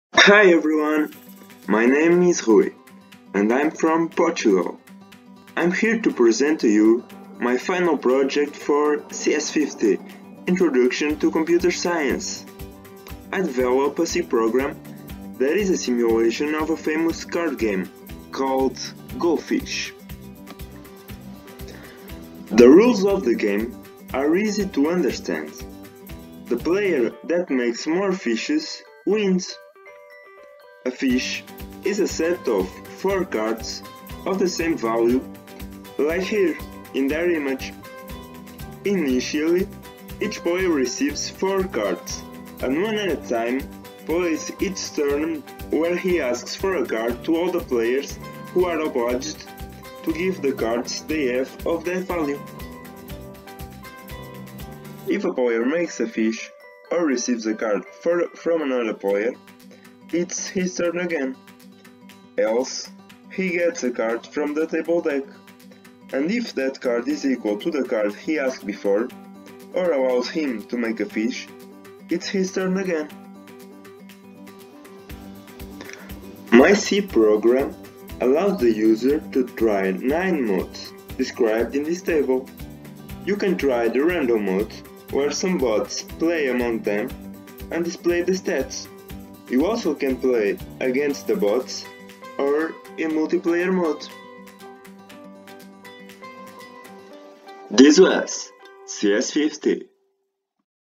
CS50 Final Project: Go Fish Card Game
This is my CS50: Introduction to Computer Science Final Project. A C program that allows the user to try 9 modes of Go Fish Card Game simultation.